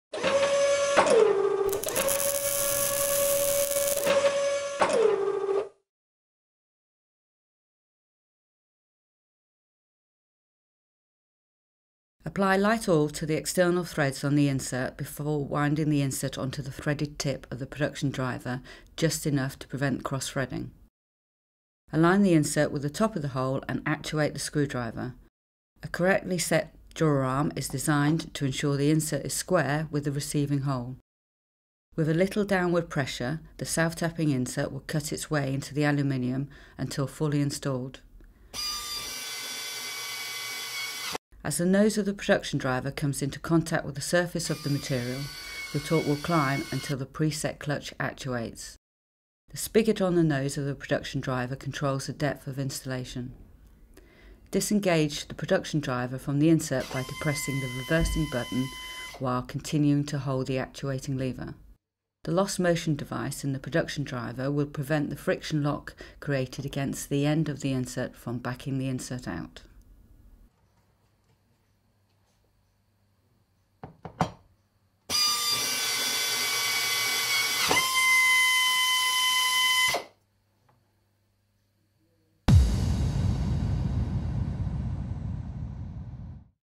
Installing A Hardened Steel M10 Thread Insert Into Aluminium Alloy with Heavy Duty Screwdriver
Instructional video showing the ease at which Tappex steel or stainless steel self threading thread inserts can be installed into Aluminium or composite materials, using the Tappex Heavy Duty DuraArm Manual Reverse Pneumatic Screwdriver.